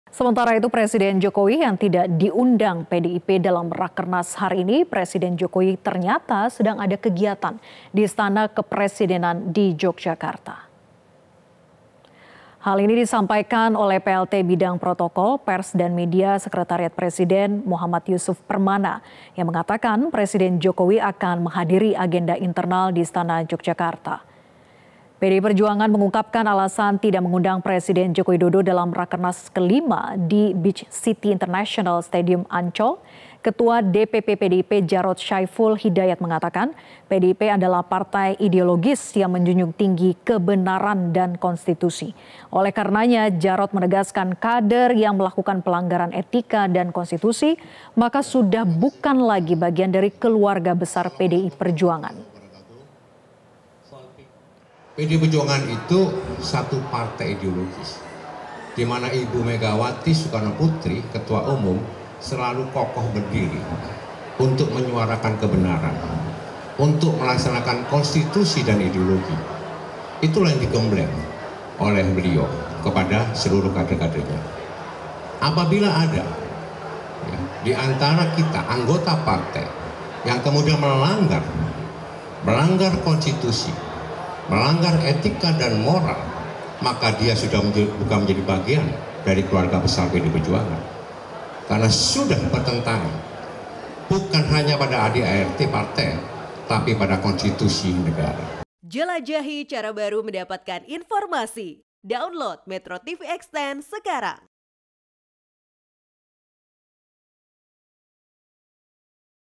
Ini Alasan Jokowi Tak Diundang PDIP di Rakernas...
MetroTV, Sementara itu, Presiden Jokowi yang tidak diundang PDIP Dalam Rakernas Jumat,(24/5/2024) Presiden Jokowi ternyata sedang ada kegiatan di  Istana Kepresidenan di Yogyakarta.